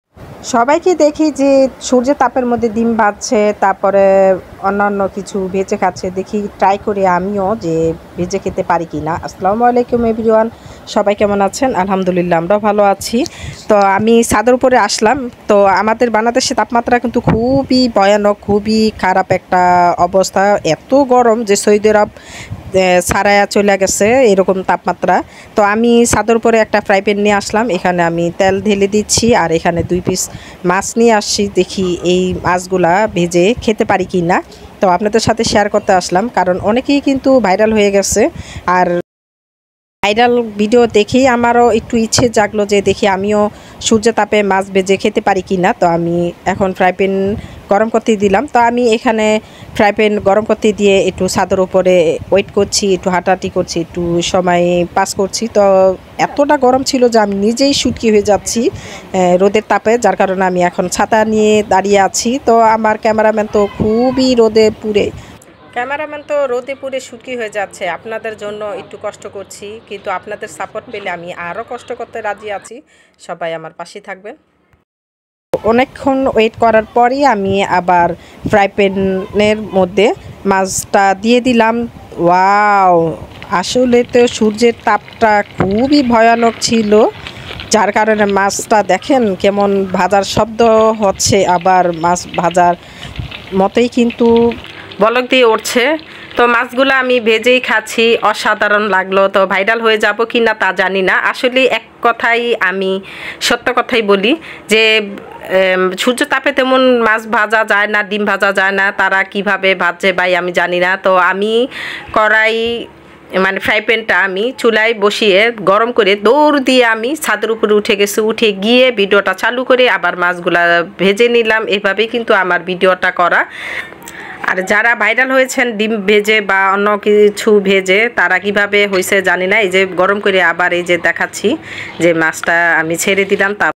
সূর্যের তাপে মাছ ভেজে নিলাম ভাইরাল ভিডিও
সূর্যের তাপে মাছ ভেজে নিলাম অসাধারণ ছিল মাছ ভাজাটা ভাইরাল হয়ে গেছি